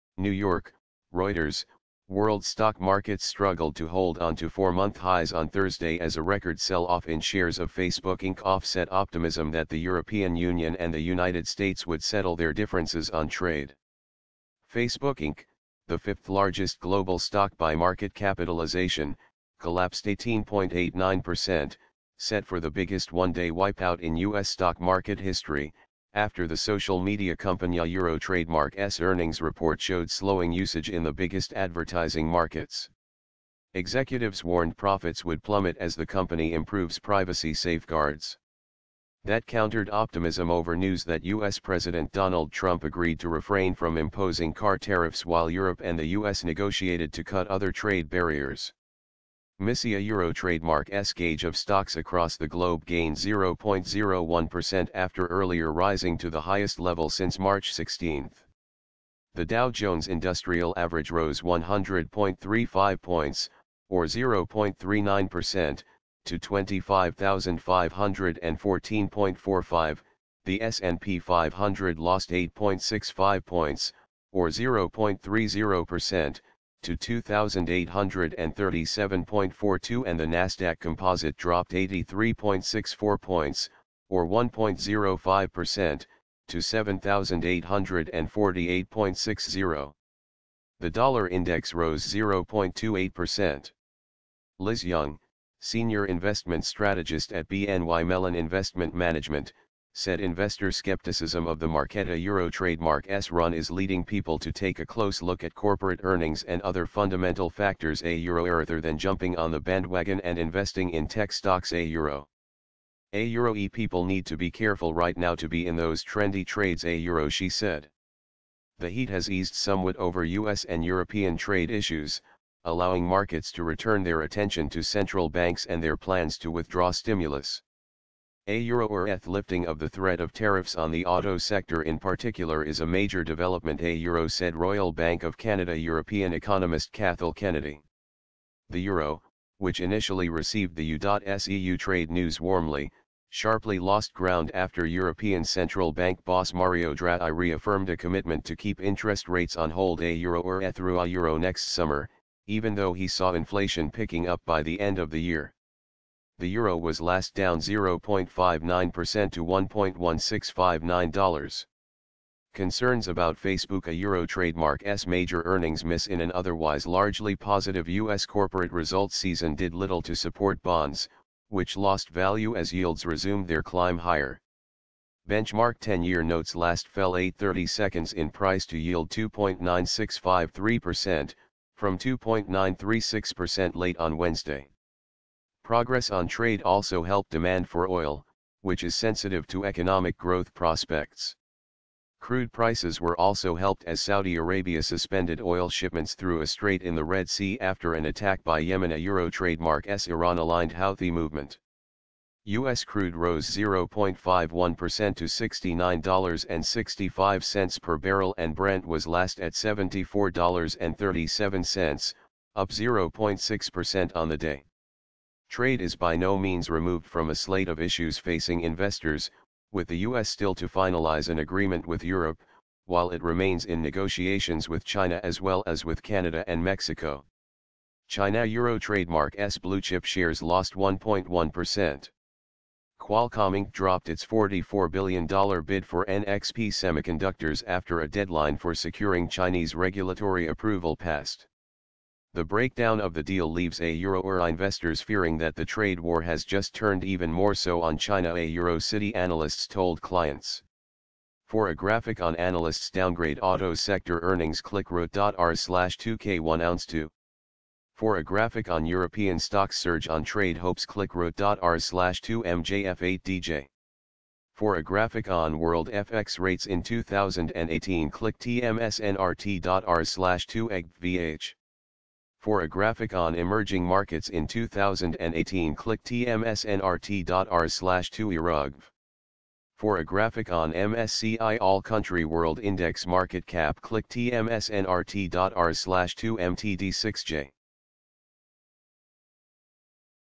Global Stocks Struggle To Hold Four-month Peak As Facebook Plunges | News Today | 07/26/2018
World stock markets struggled to hold on to four-month highs on Thursday as a record sell-off in shares of Facebook Inc offset optimism that the European Union and the United States would settle their differences on trade.